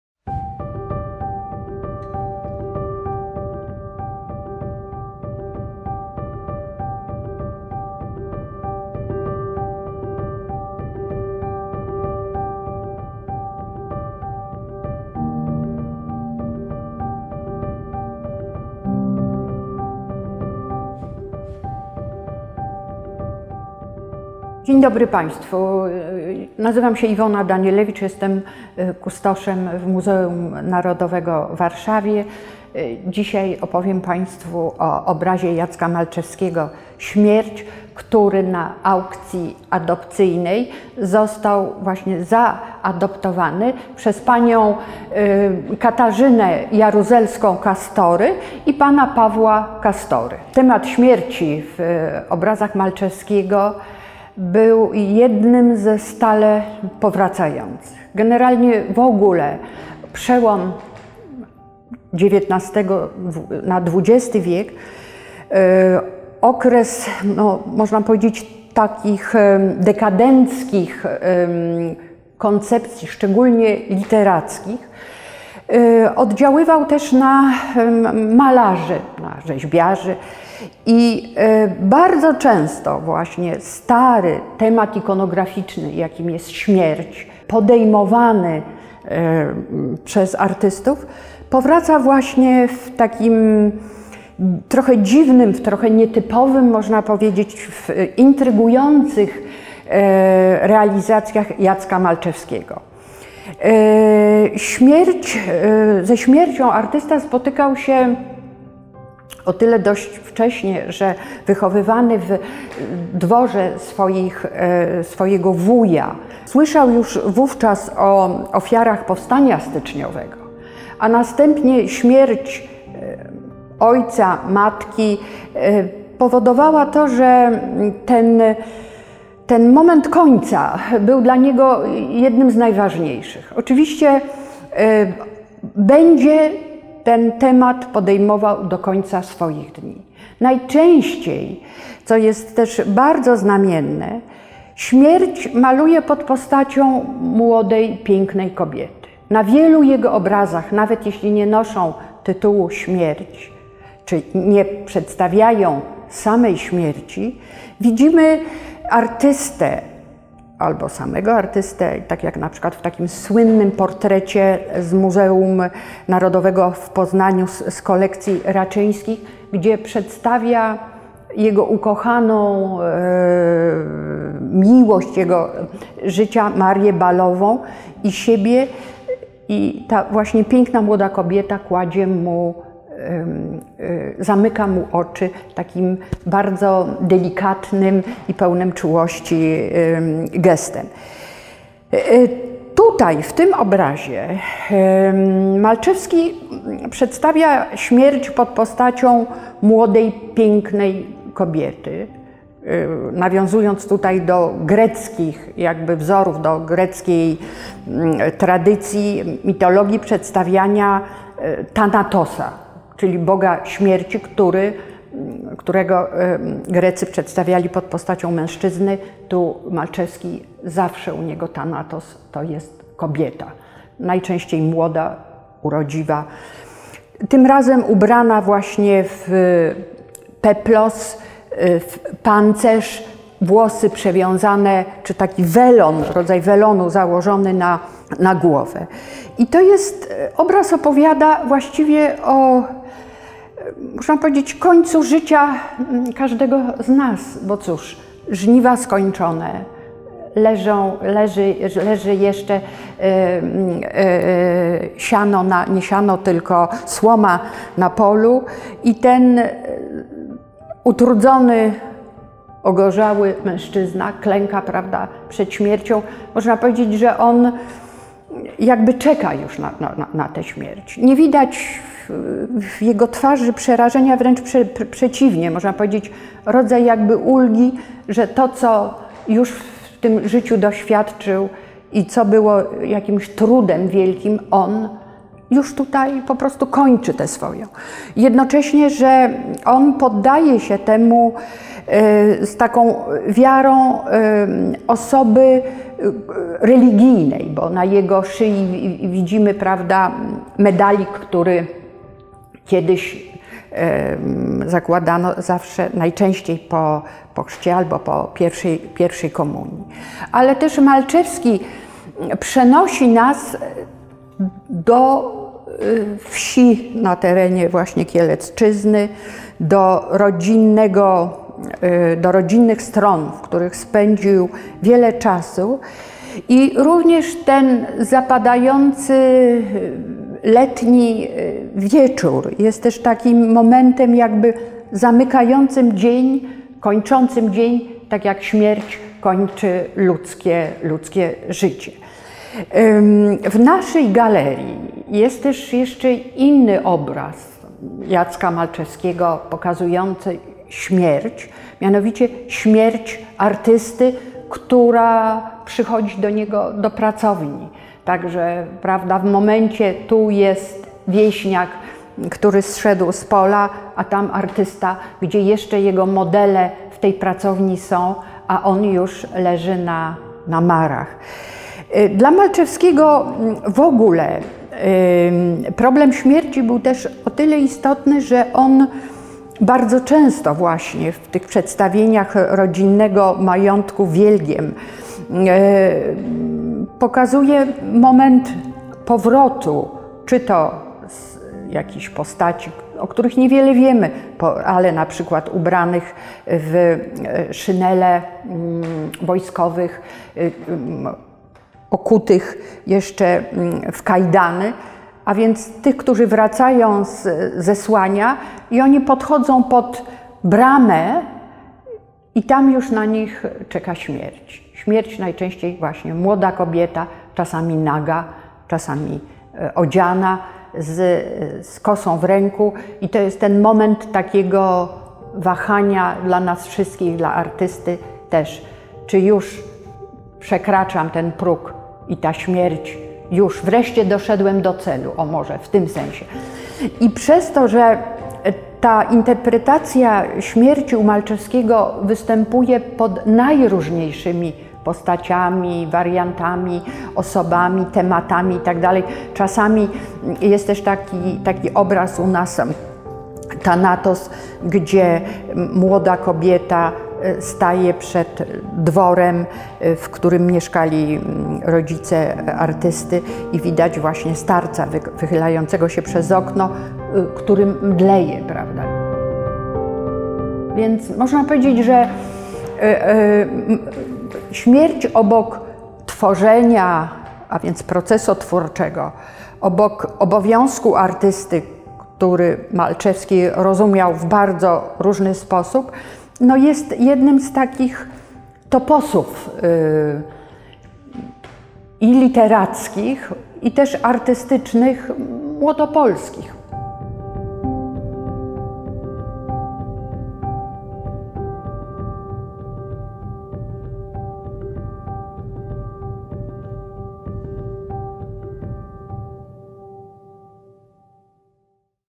Bliżej Sztuki #9 | Jacek Malczewski
Różne oblicza Thanatosa

W dzisiejszym odcinku „Bliżej Sztuki” Iwona Danielewicz opowie o wizerunkach śmierci w twórczości Jacka Malczewskiego.

Projekt „Bliżej sztuki", którego partnerem jest MNW, organizuje Stowarzyszenie Przyjaciele Muzeum Narodowego w Warszawie dzięki środkom z programu „Kultura w sieci" Ministerstwa Kultury i Dziedzictwa Narodowego oraz Narodowego Centrum Kultury.

Obraz „Śmierć” Jacka Malczewskiego zaadoptowali Państwo Katarzyna Jaruzelska-Kastory i Paweł Kastory.